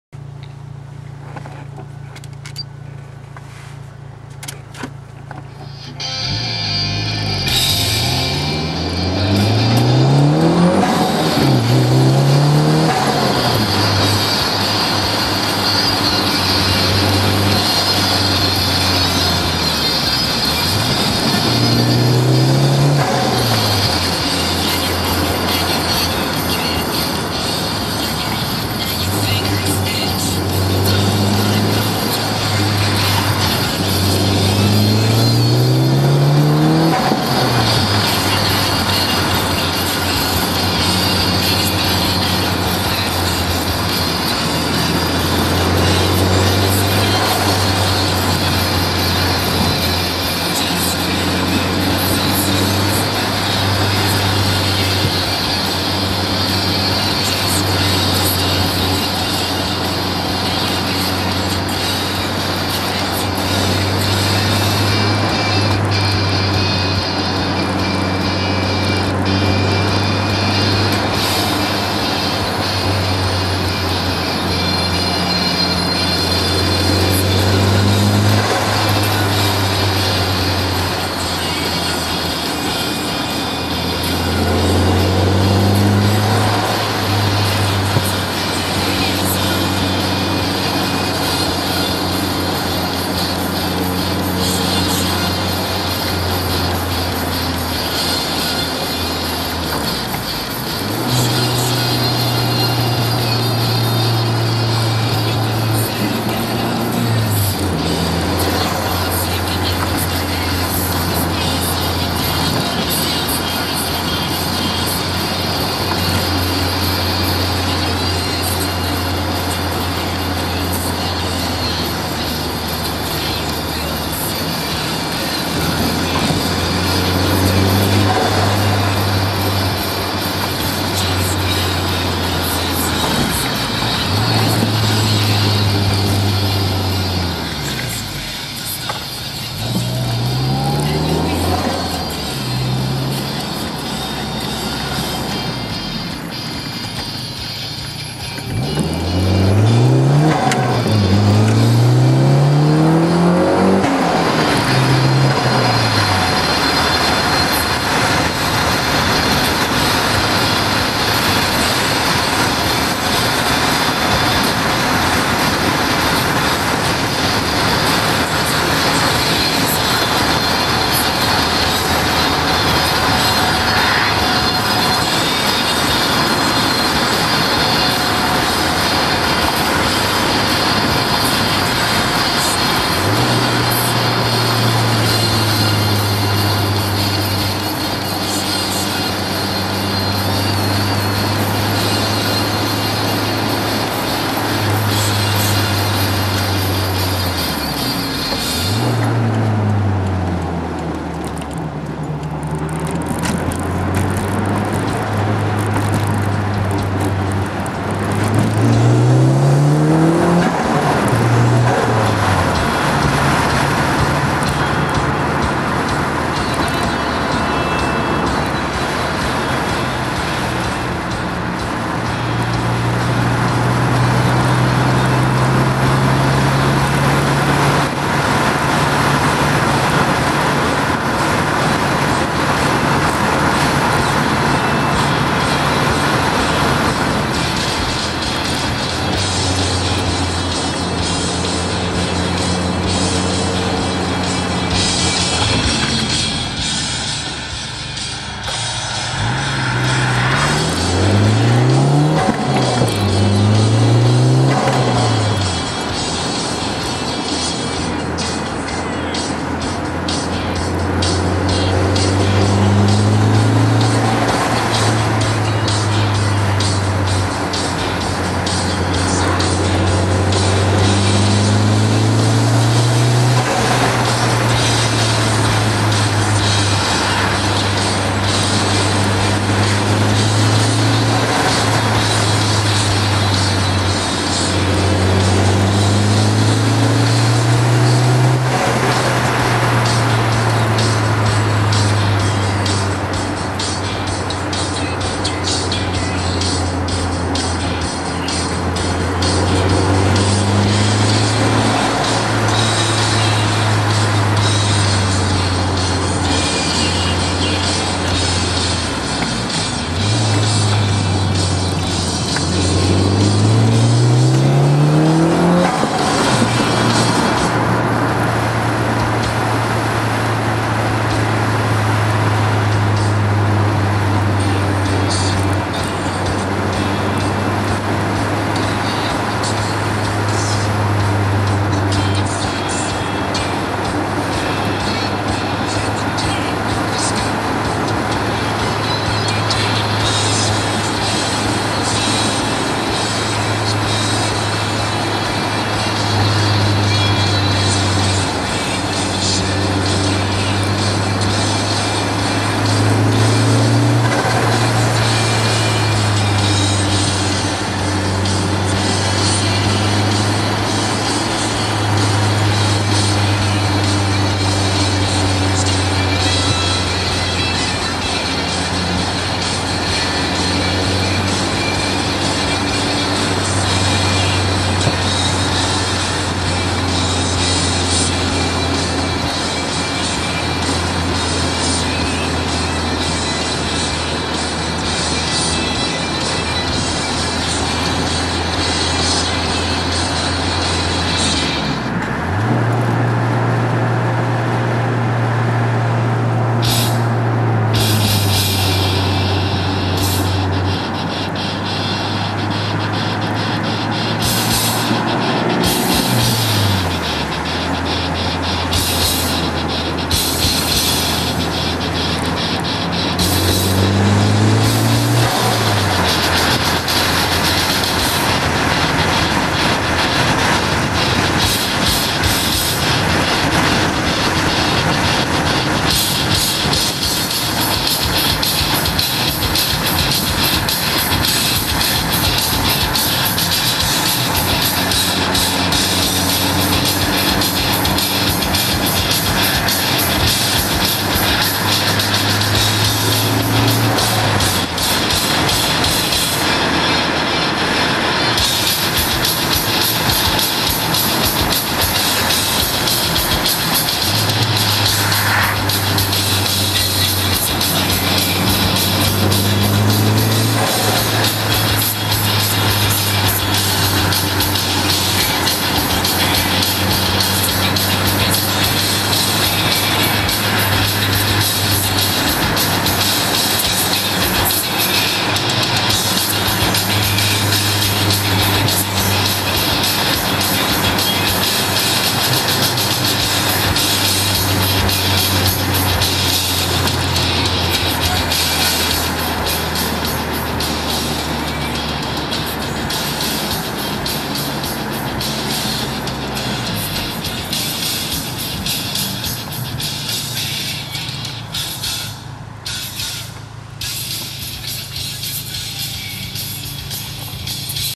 Typisk rånetur
Går ikke an å kjøre over fartsgrensa i juli nei.Møtte 2 biler på feil side av veien på denne lille turen(cameratest/kvalitet).